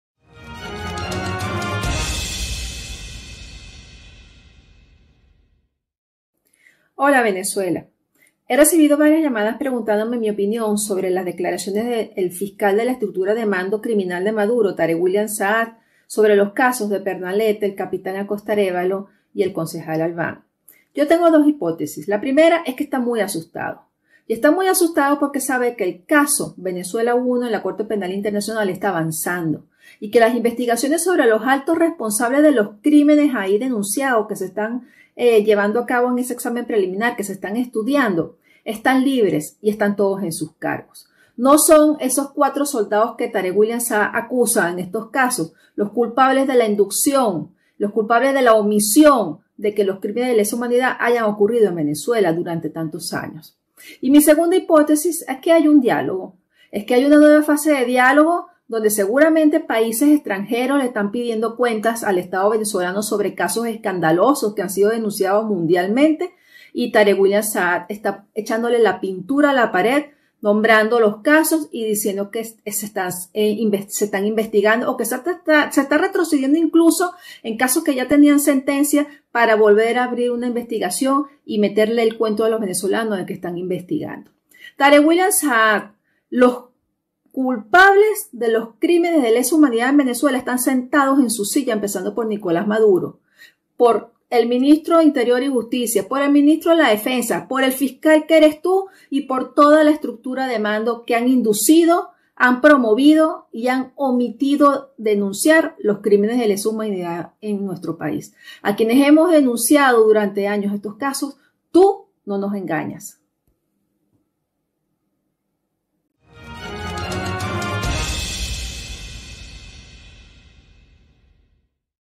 RESPUESTA TAMARA SUJU A DECLARACIONES DEL FISCAL TAREK WILLIAM SAAB
Abogada Tamara Suju